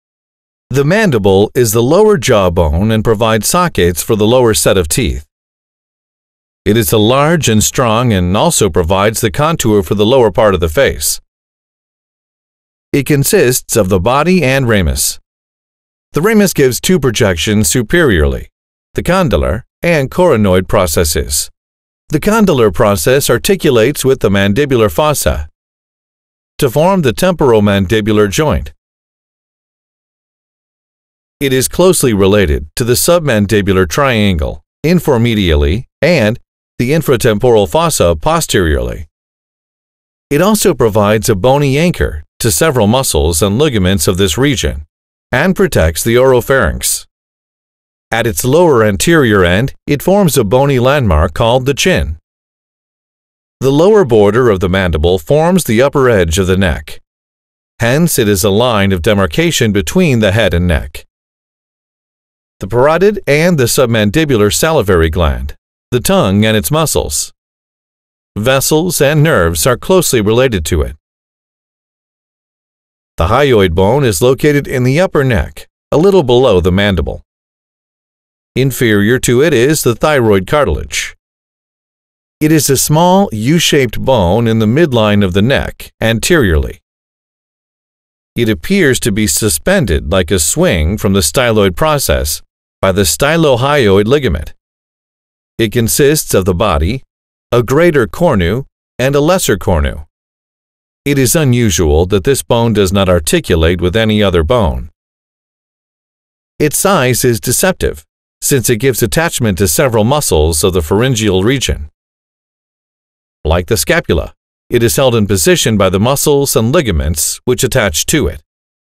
Mandible and hyoid bone - Head & Neck
Mandible forms the lower part of skull while hyoid bone forms the upper part of neck, below the madible. Being large and strong, the mandible functions as a bony anchor to several muscles and ligaments, and is closely related to the salivary glands, major vessels and nerves of face. The U shaped hyoid bone is suspended below the mandible by the stylohyoid ligament. Anatomical features of the mandible and hyoid bone, structures and parts associated with them, and clinically important diseases/disorders are discussed in this 3D audio-visual presentation. The presentation is also accompanied by images with pop-out descriptions for each label for easy learning and comprehension.